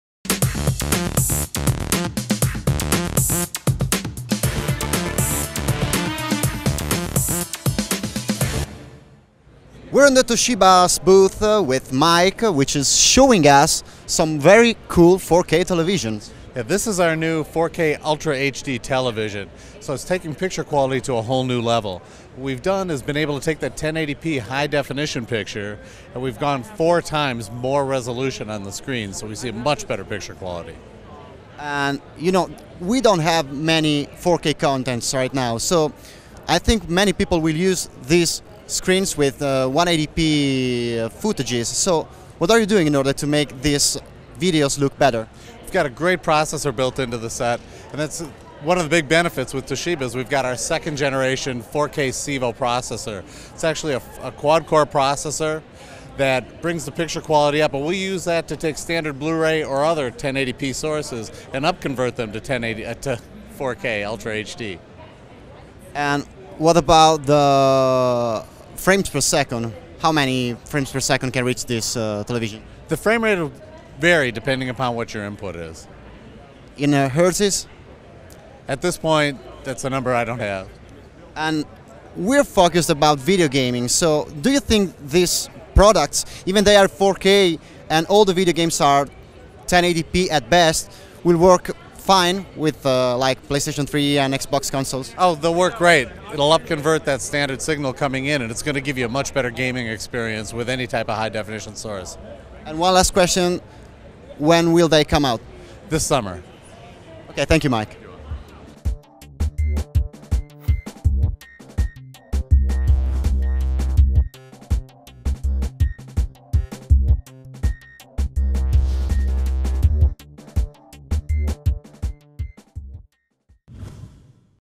CES 13: Toshiba 4K TV Interview
Toshiba is showing us its 4K screens, with a powerful upscaler that allows users to watch 1080p contents on these UHD TVs with some noticeable improvements.